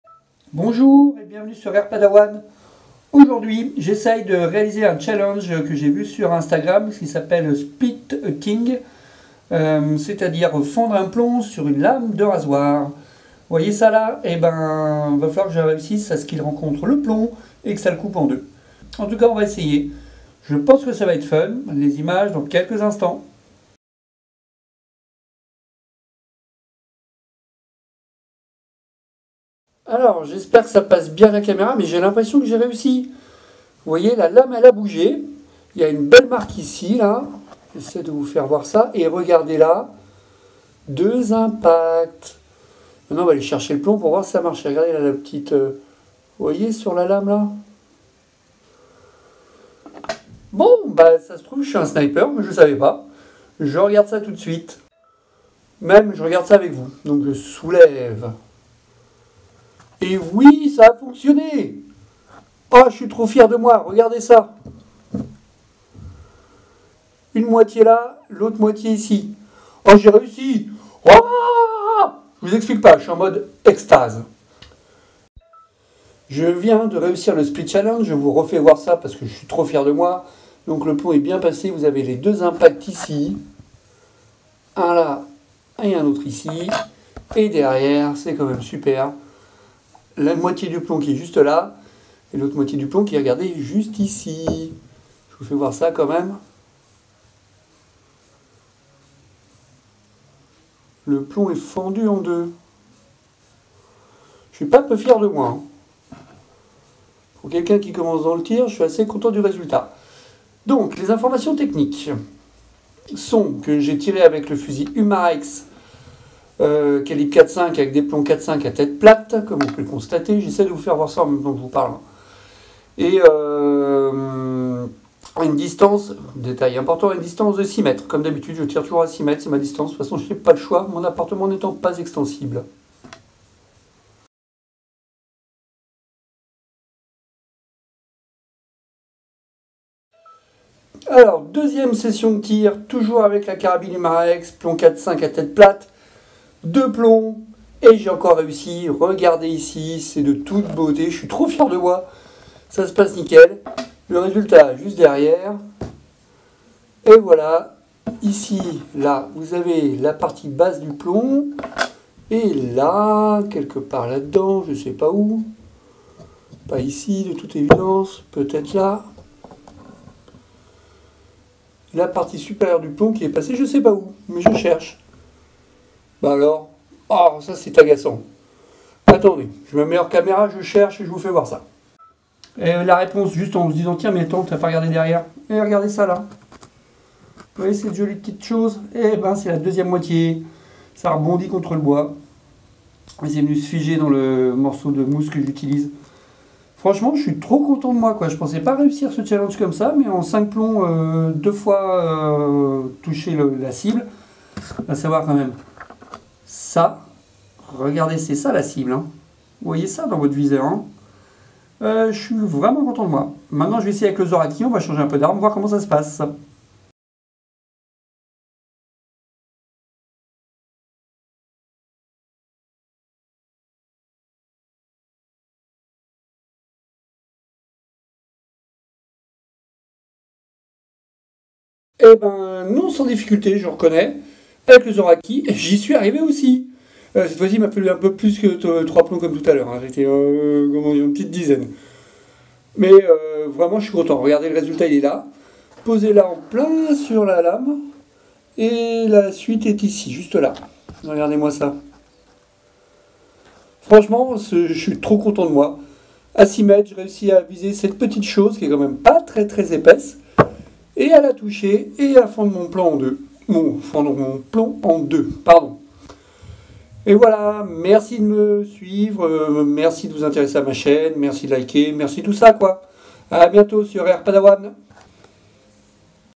SPLIT A TING - RAZOR BLADE
Bonjour les gens,

aujourd'hui je tente le challenge "SPLIT A TING ON A RAZOR BLADE"

Vu sur Instagram =

et j'ai réussi !

Tir à 6m
Umarex nxg apx plombs 4.5mm tête plate
Zoraki hp01-2 ultra 5.5mm

Visiter mon Instagram =